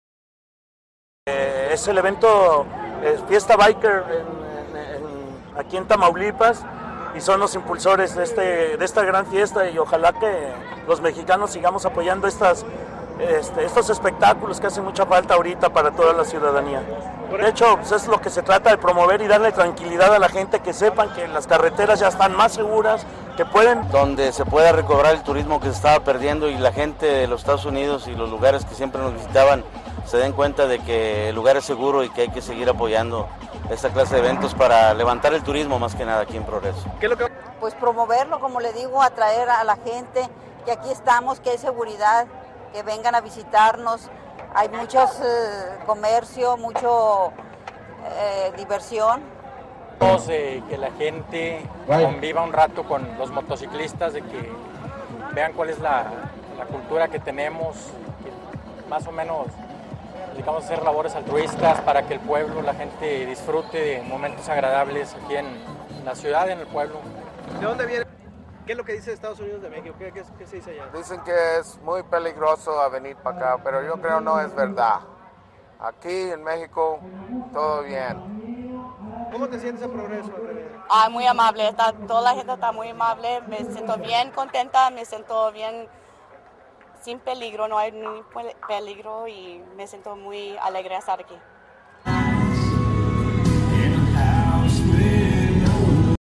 Celebran el "Bike Fest" la Fiesta de los Motociclistas en Progreso Tamaulipas.
Con la participación de motociclistas de varios estados de la republica mexica  y de los Estadios Unidos se llevo a cabo en la Villa de Nuevo Progreso Tamaulipas el  "Bike Fest"
Con este evento en donde hubo acrobacias en motocicleta se trata de volver a incentivar la confianza de los turistas.
Los participantes de procedentes de los estados unidos confirmaron su confianza en seguir visitando  México y en especial Progreso Tamaulipas.
Más de trescientos motociclistas se reunieron a festejar este evento en donde el atractivo principal fue el escuadrón de acrobático de la confederación de motociclistas, oficiales y policías de caminos que vinieron desde la ciudad de México.